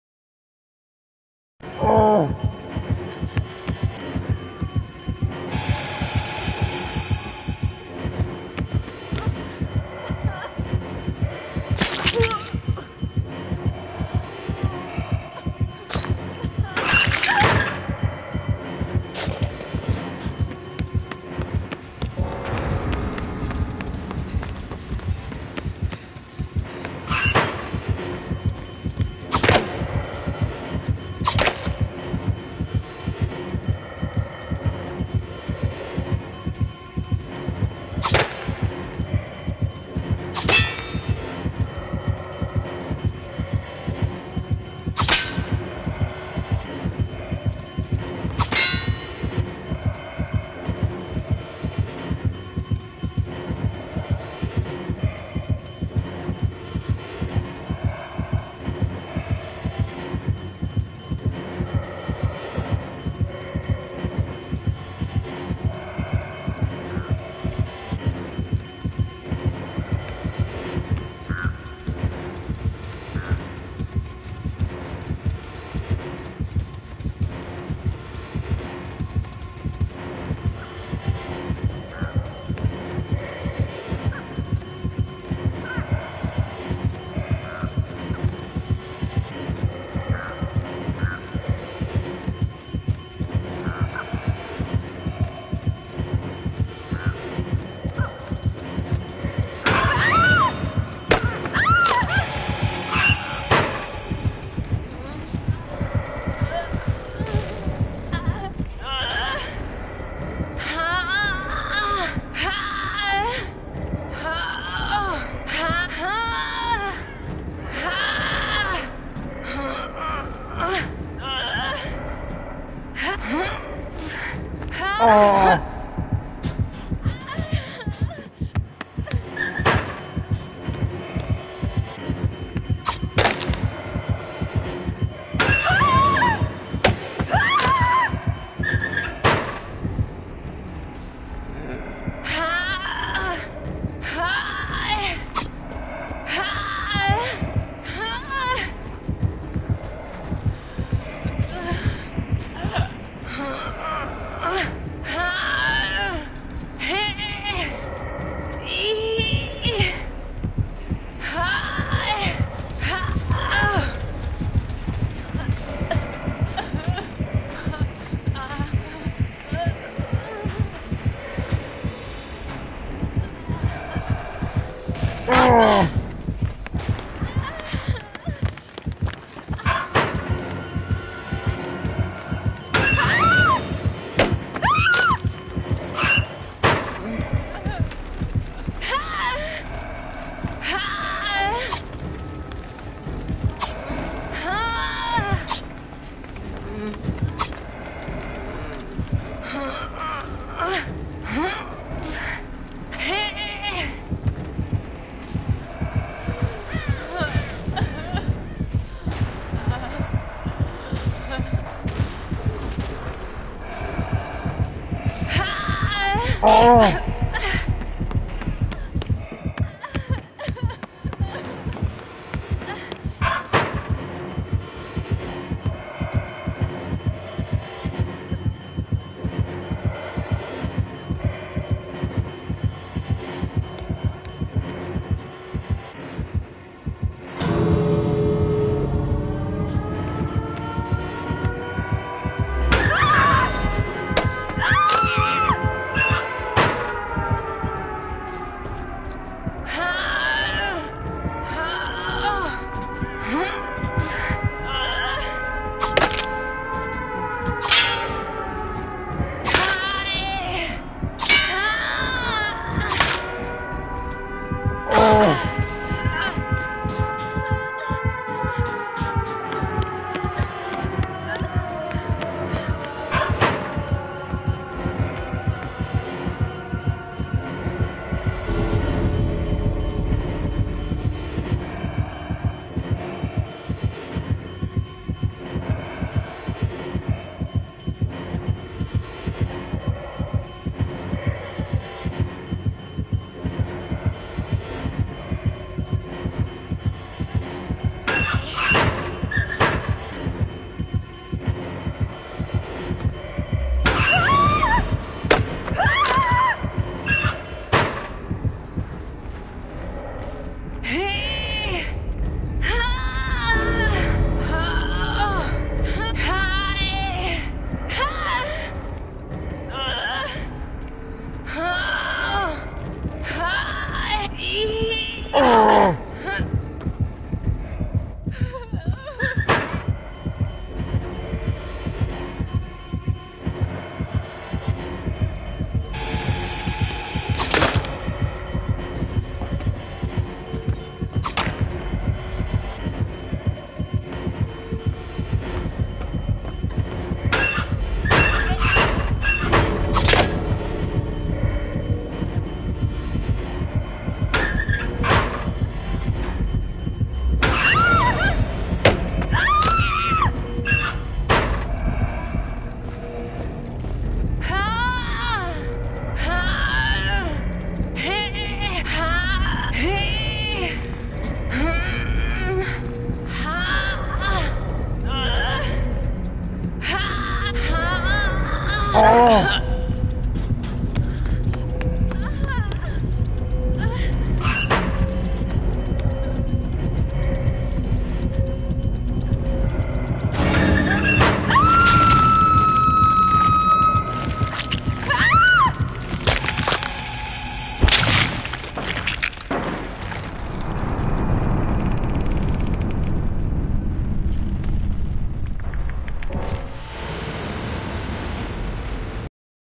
6 minutes and 50 seconds of a Myers trying to mori me😂
This Myers tried to mori me this whole time.

Follow Twitch.tv/PoizinYT